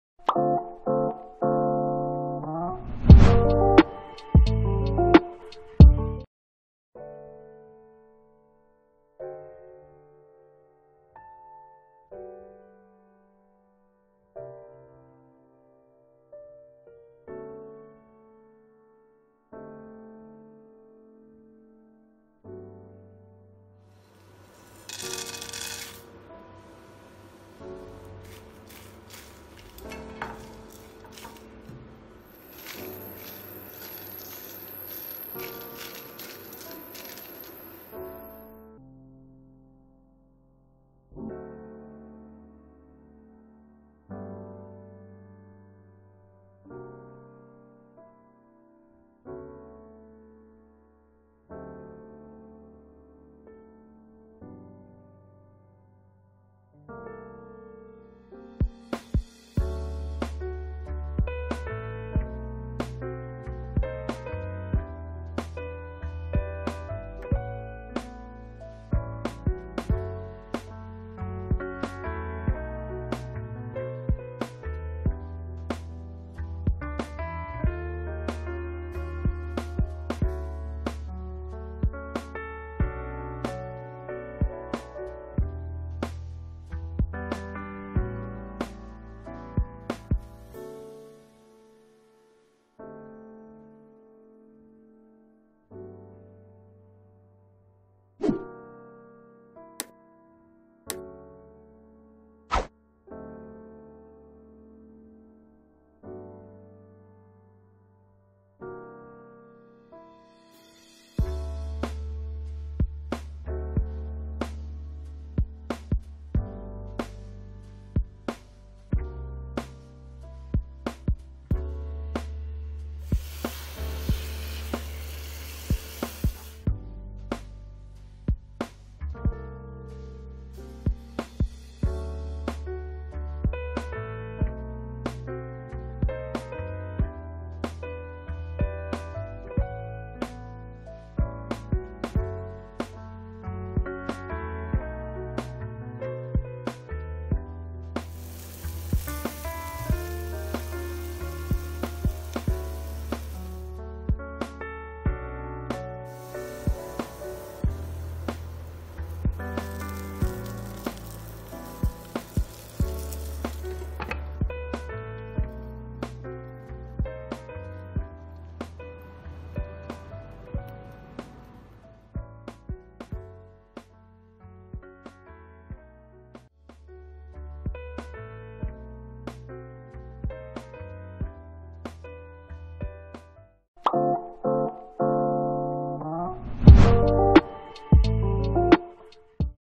Keema Recipe
Learn to make this amazing smoked dish through super easy steps.